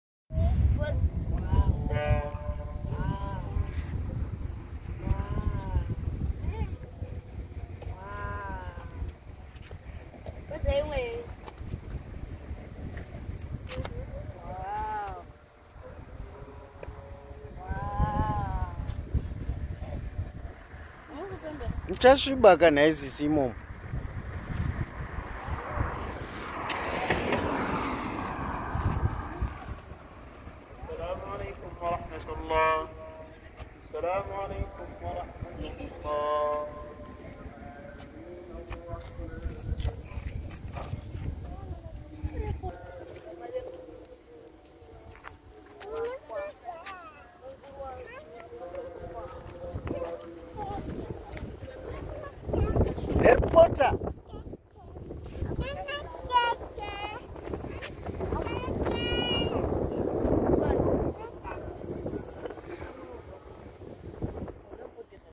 Hailey Juanita Izwirashe Ndawana with siblings this motning in Al Thakira, Qatar.
05 January 2024.

SK Ndawana Snr, Mai Praise Kim, Mudiwa Ndawana, Hailey J I Ndawana on a stroll this morning.